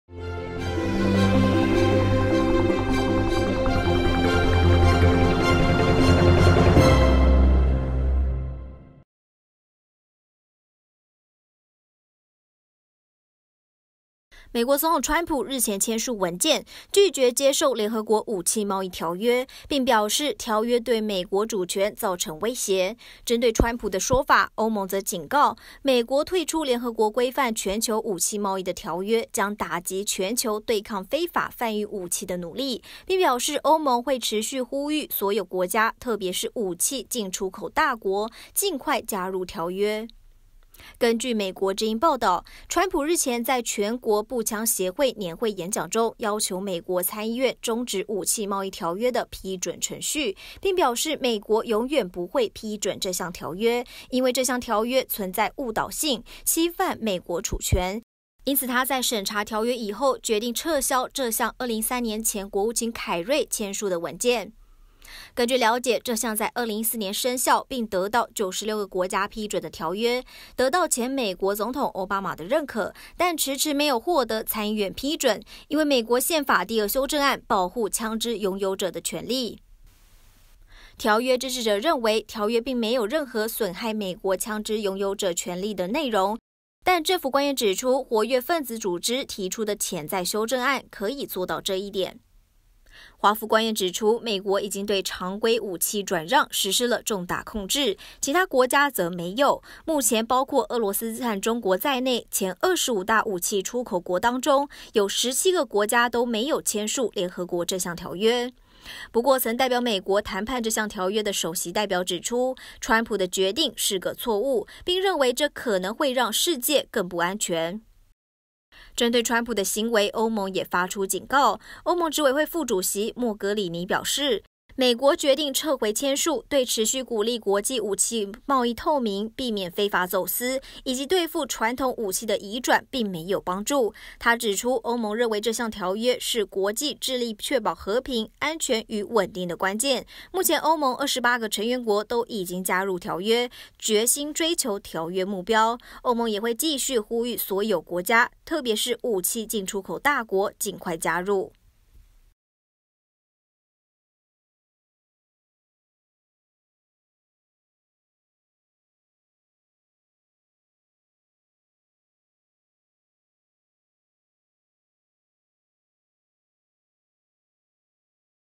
“侵犯美国主权”，川普退出联合国武器贸易条约｜新闻时时报（20190428）
美国总统川普日前签署文件，拒绝接受“联合国武器贸易条约”，并表示条约对美国主权造成威胁。针对川普的说法，欧盟则警告，美国退出联合国规范全球武器贸易的条约，将打击全球对抗非法贩运武器的努力，并表示欧盟会持续呼吁所有国家，特别是武器进出口大国，尽快加入条约。